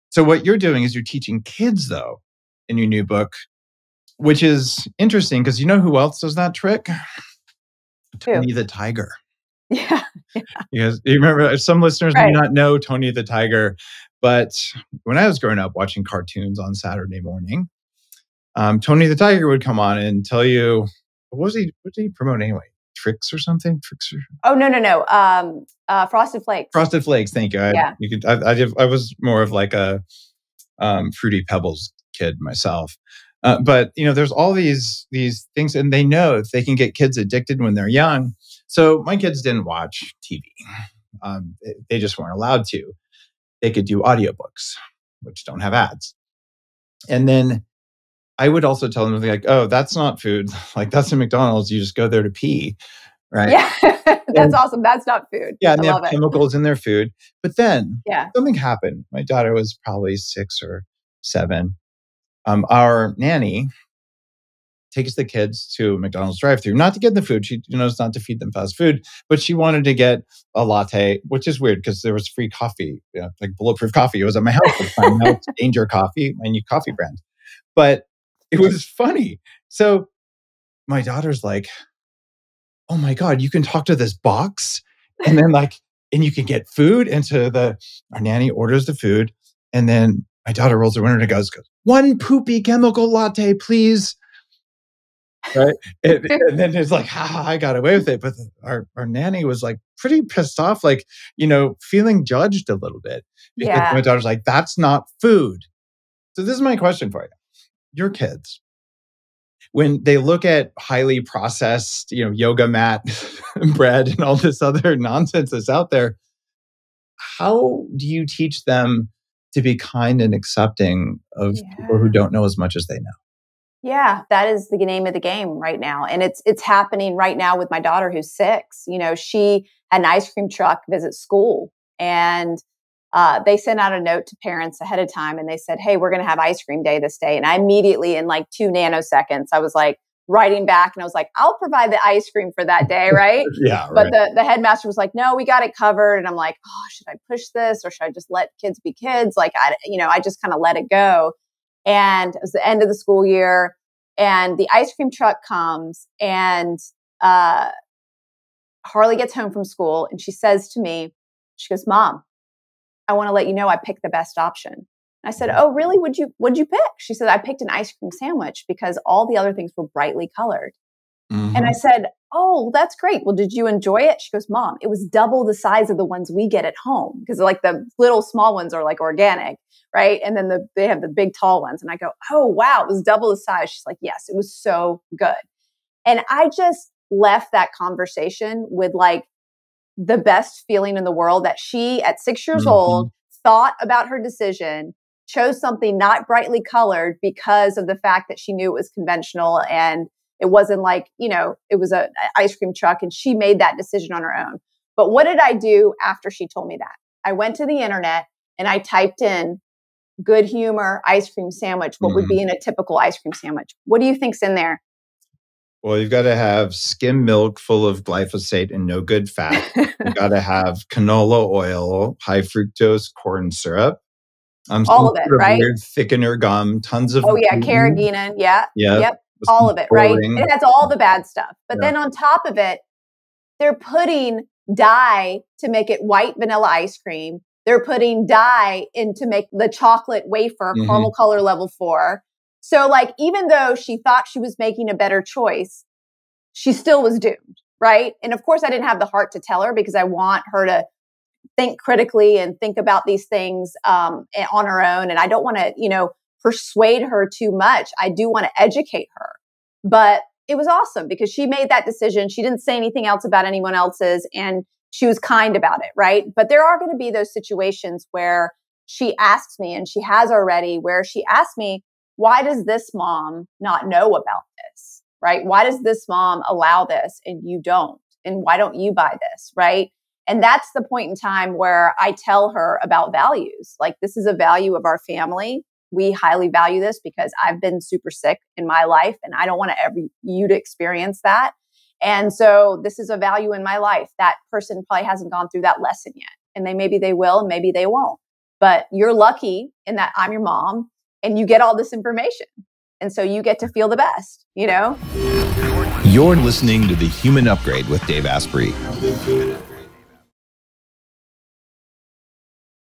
Teaching Kids to Make Smart Food Choices with Vani Hari | Dave Asprey
In this conversation we discuss the importance of educating your kids on processed foods and proper nutrition early on. Vani and I share some of the lessons we’ve learned in teaching our kids about nutrition, and how we’ve empowered them to choose foods that make them feel good.